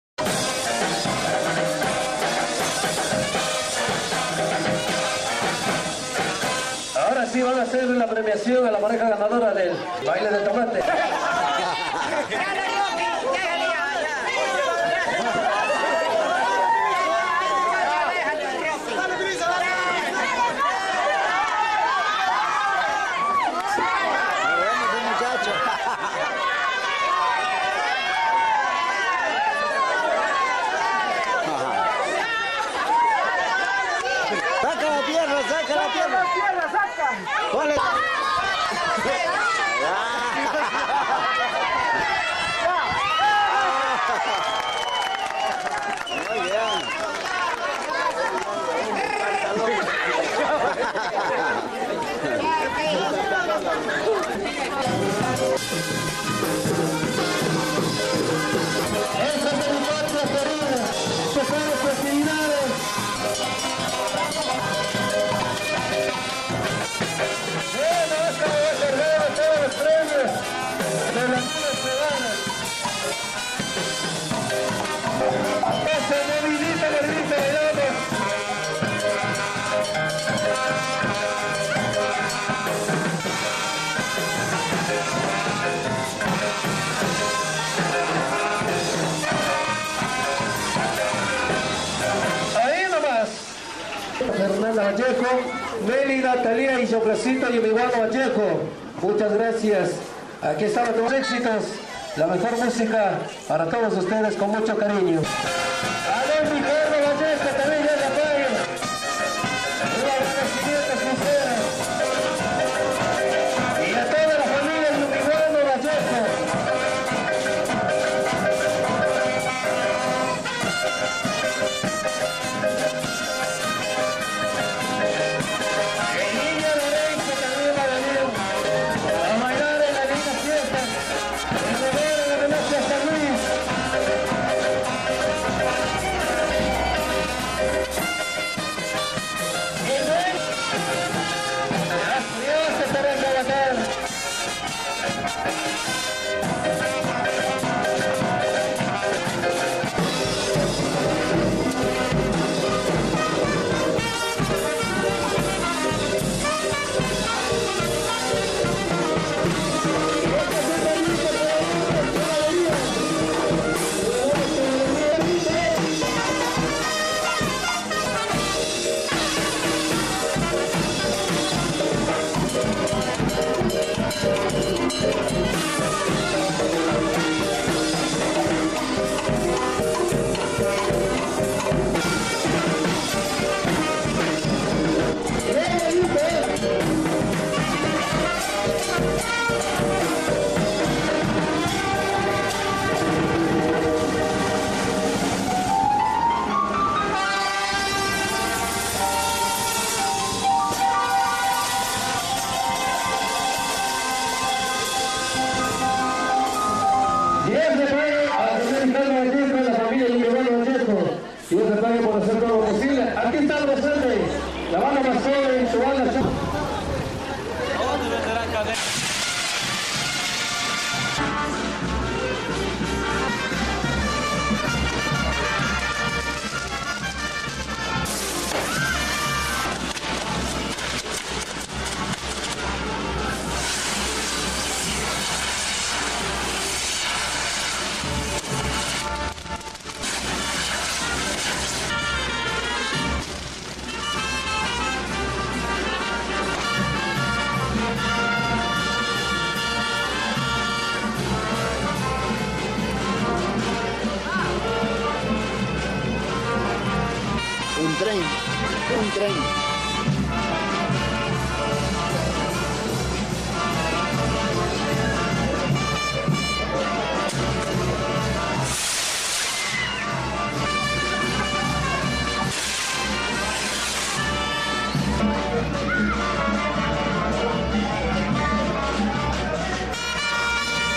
GUASUNTOS VIVE - ECUADOR 2007
el tradicional palo encebado, y la infaltable banda de musicos